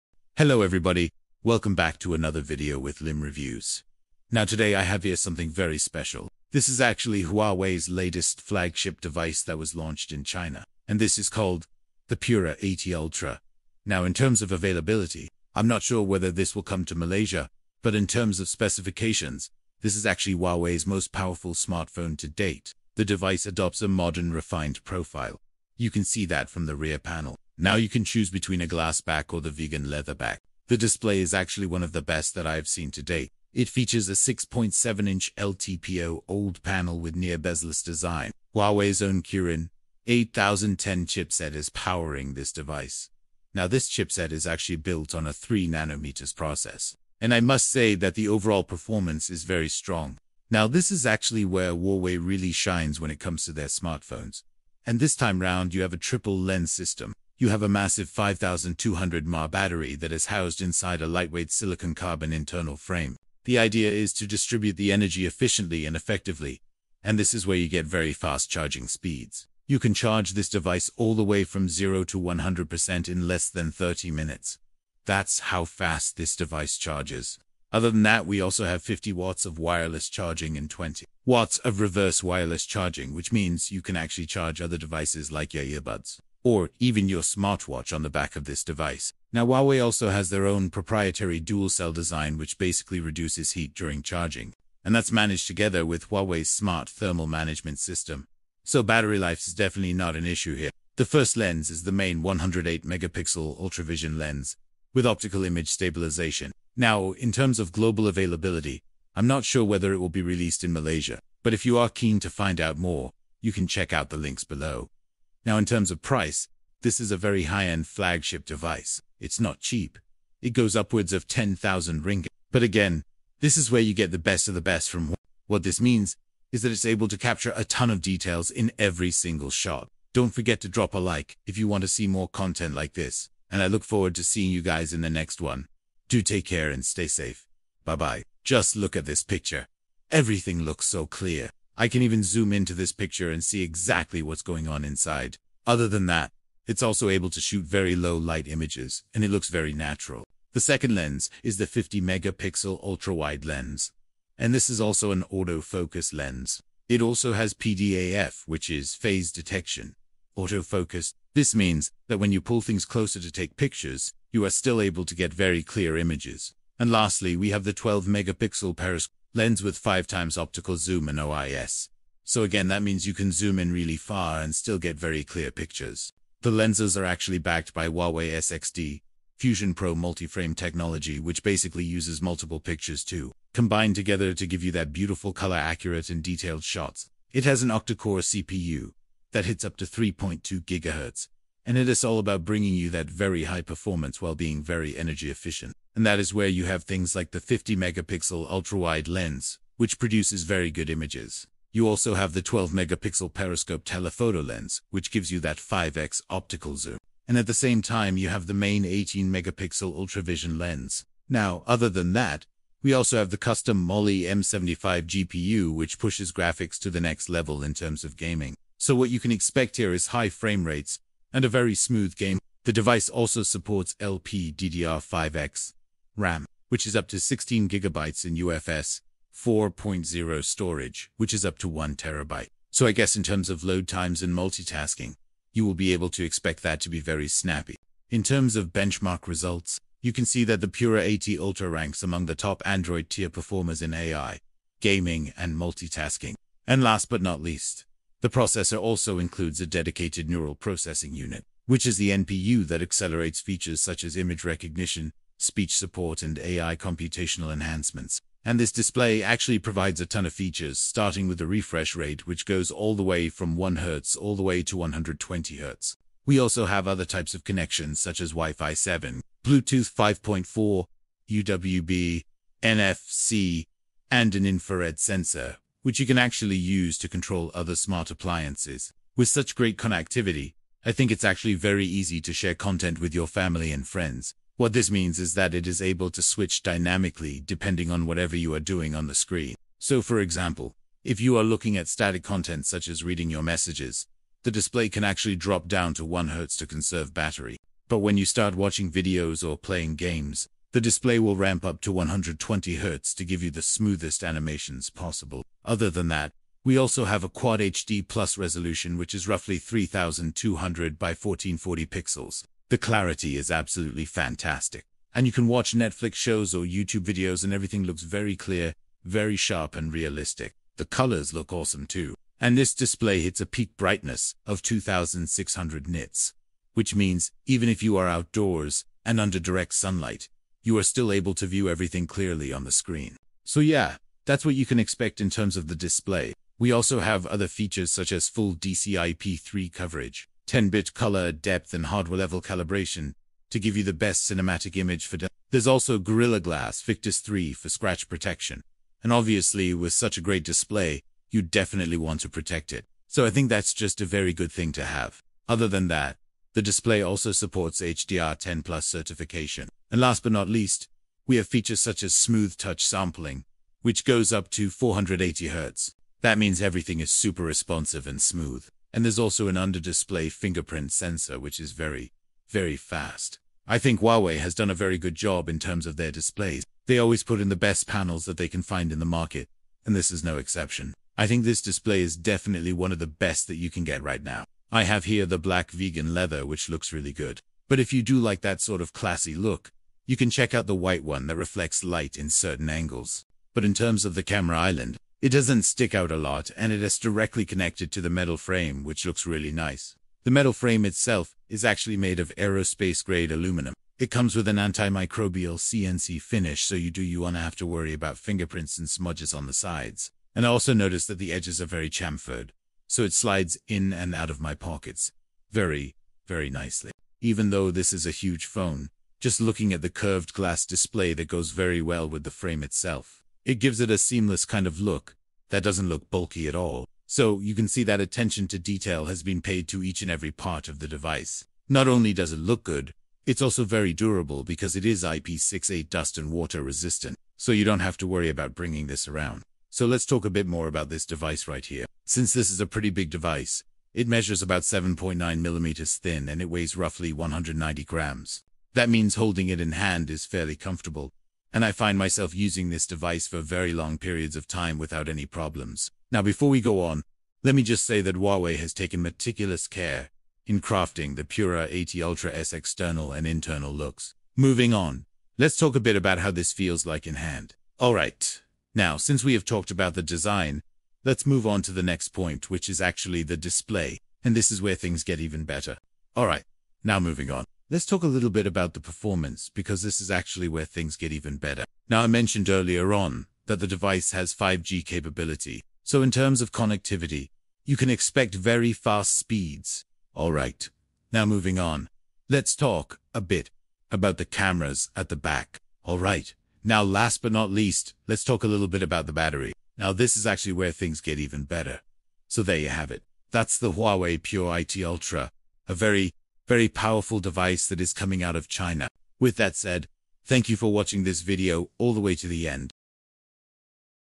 HUAWEI Pura 80 Ultra: HOW Is This Possible?
Discover the Huawei Pura 80 Ultra in this in-depth review covering its design, display, performance, camera innovations, battery life, AI features, and HarmonyOS NEXT. Explore why it stands out as Huawei's most premium smartphone of 2025.
Get a complete understanding of Huawei’s boldest phone yet—the Pura 80 Ultra. From its cinematic display and pro-level camera system to its futuristic AI features and HarmonyOS NEXT experience, this phone isn’t just high-end—it’s ahead of its time. If you're a tech lover, content creator, or power user, this is the phone worth knowing.